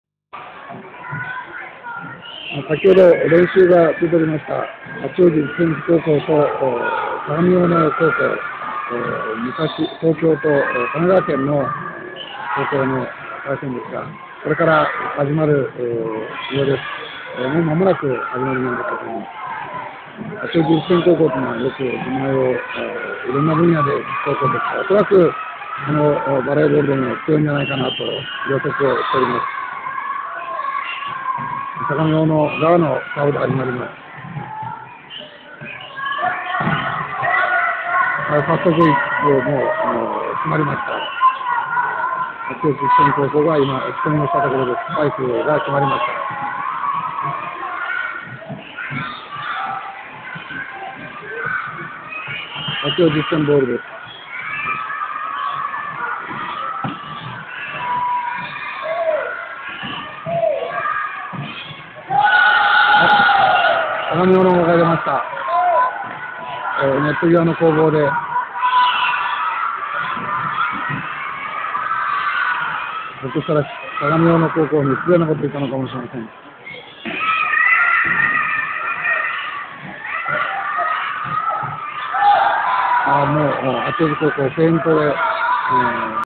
八王子実践女子バレー部の力、武相大会開催08．8.25
八王子実践高校女子バレー部の力を見ました。高校バレーボール武相大会の模様ですが、町田市立総合体育館で開催されたものです。町田市バレーボール連盟。相手は相模大野でしたが、私には力の差が感じられました。明日も試合があります。08．8.25　記：吉田つとむ